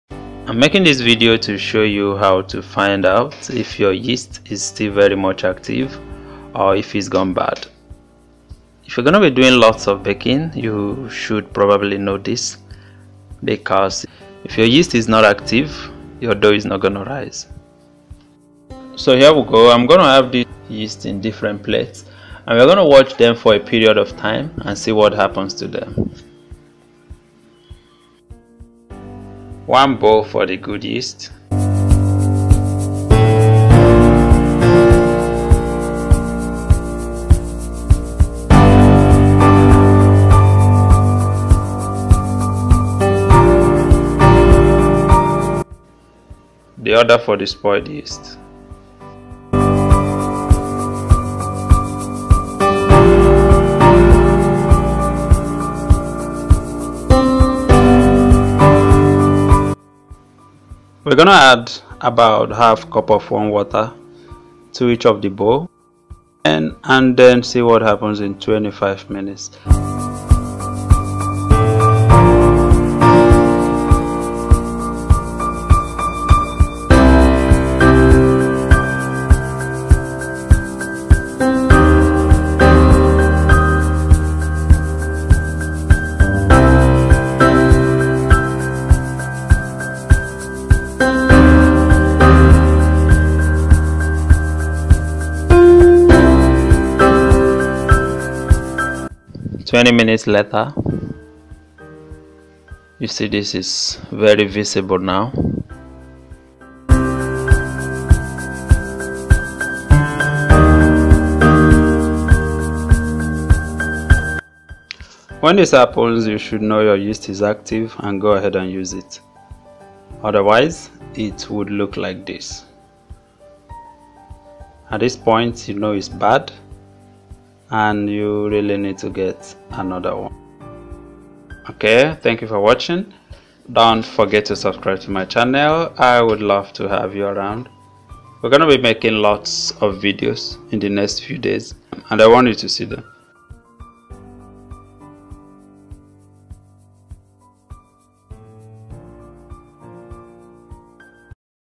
Good Yeast, Bad Yeast - The experiment
Good or active yeast would make your dough to rise normally. If your yeast is bad, well your dough wouldn't rise this video shows you how to find out if the yeast is good or bad before you even start baking.